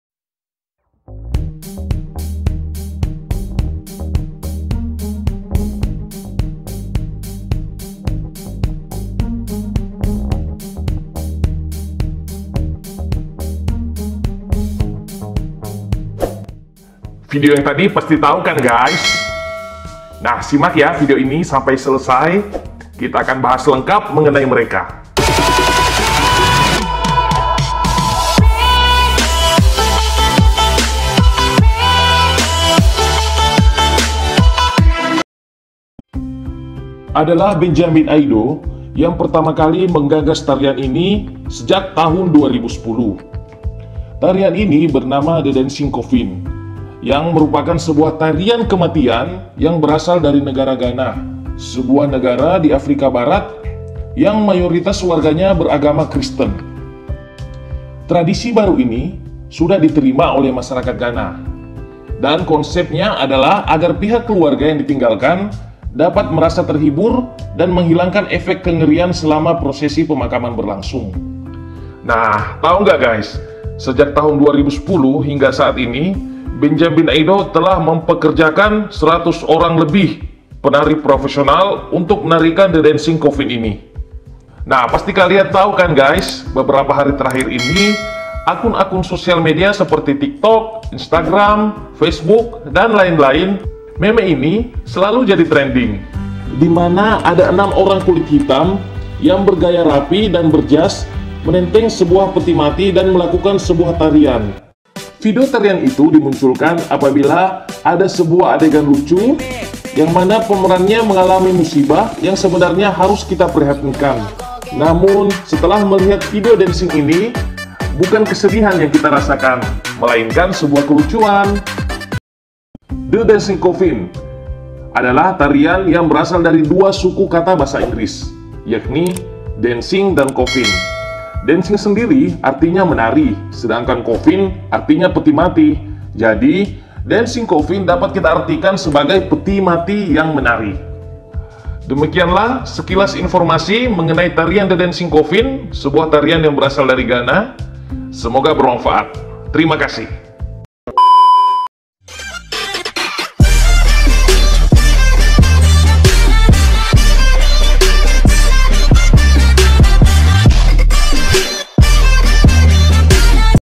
MEME Coffin Dance, TARIAN PETI MATI
Coffin Dance merupakan tarian yang dilakukan oleh sekelompok orang yang berpakaian jas rapi, sambil membawa peti mati.